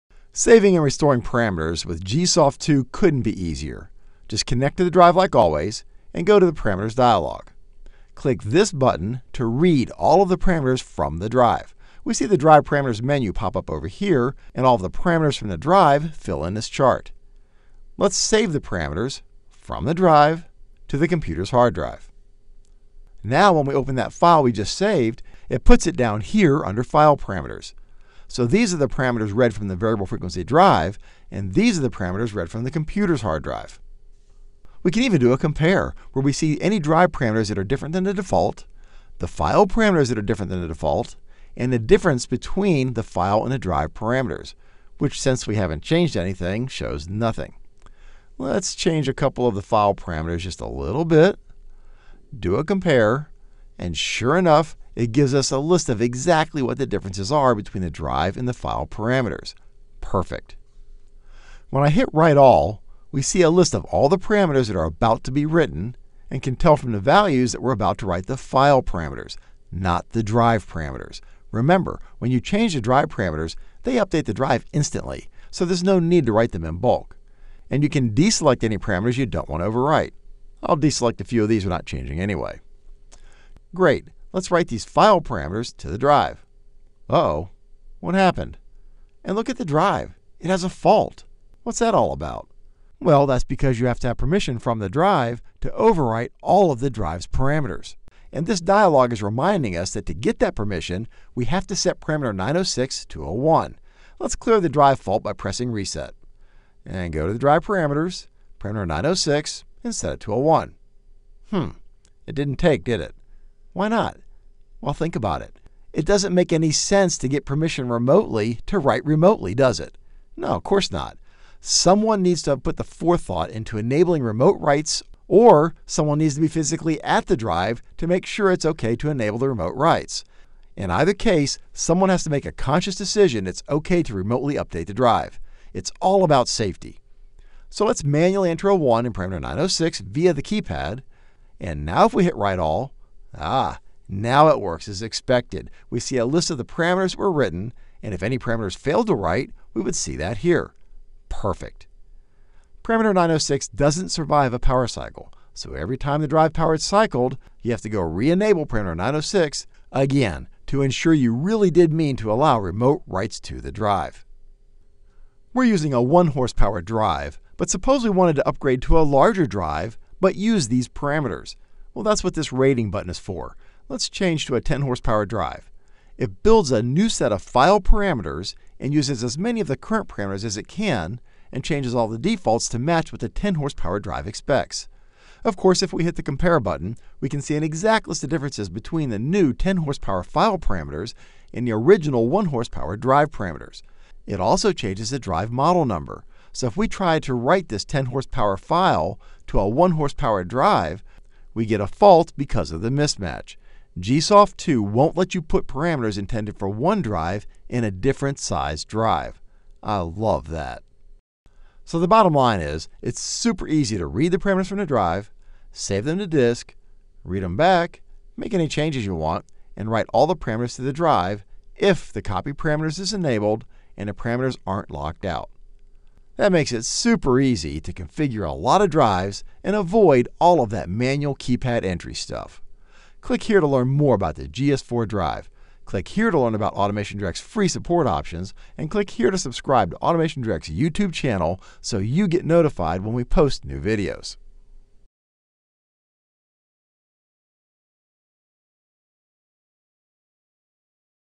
DURApulse GS4 VFD GSoft2: Part 3 - Backup & Restore Parameters from AutomationDirect
Backing up and restoring parameters with the GS4 Variable Frequency Drive (VFD) is super easy with the GSoft2 Application which you can download for FREE from the AutomationDirect.com website. Learn how in this demo/tutorial.